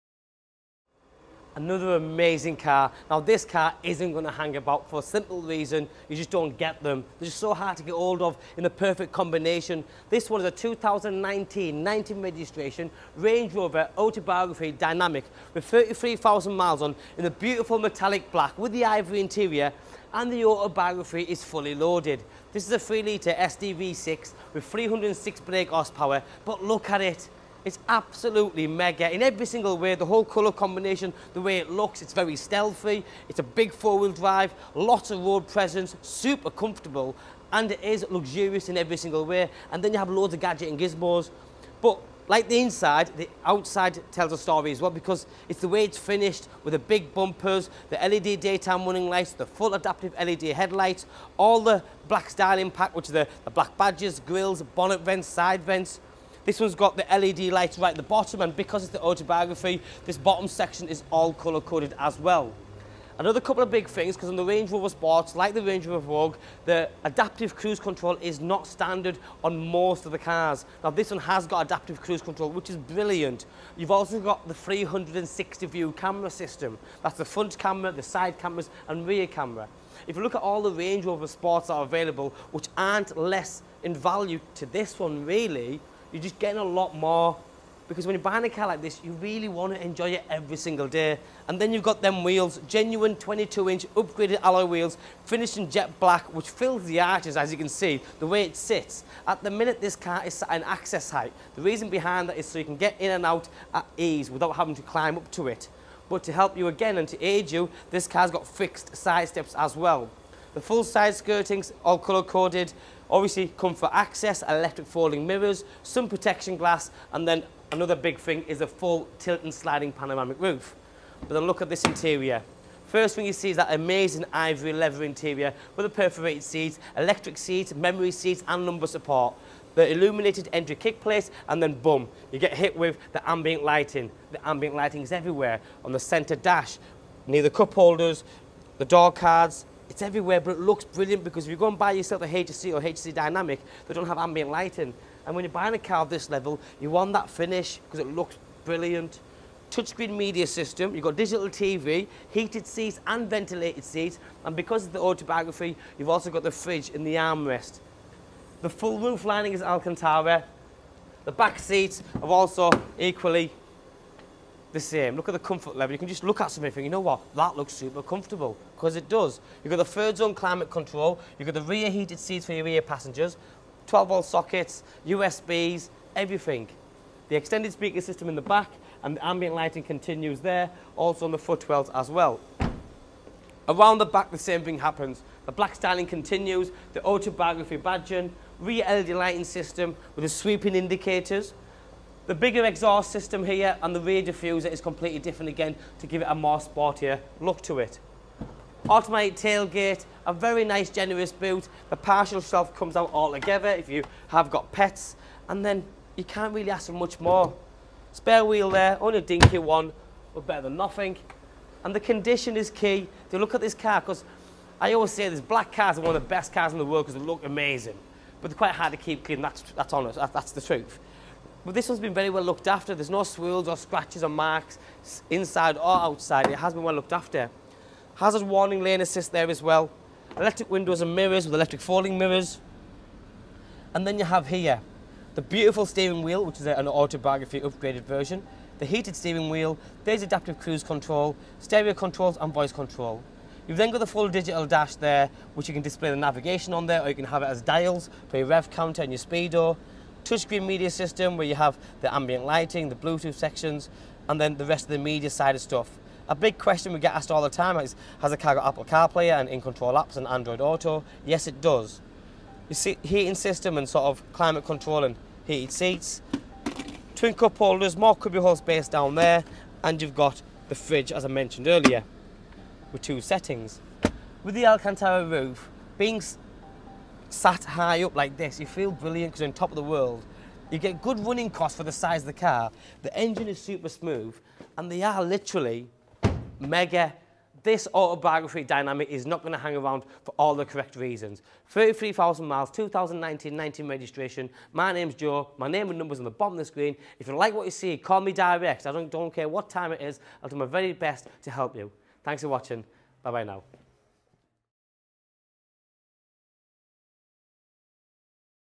2019 Range Rover Sport Autobiography Dynamic
At the height of the RR Sport line up, the Autobiography is a true showcase of the luxury sport SUVs potential. Finished in the most sought after stealth exterior with a luxurious Ivory interior